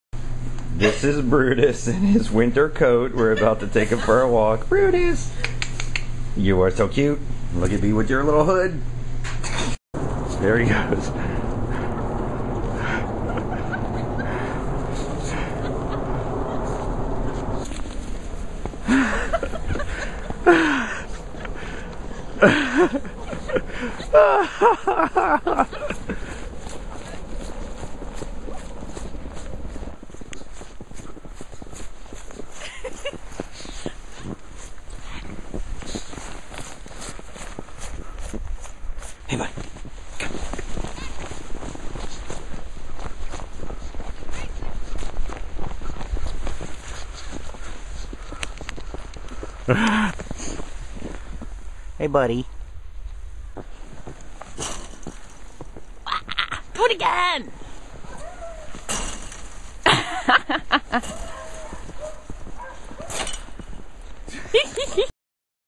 Snow day with Brutus
Our pug Brutus just loves the snow.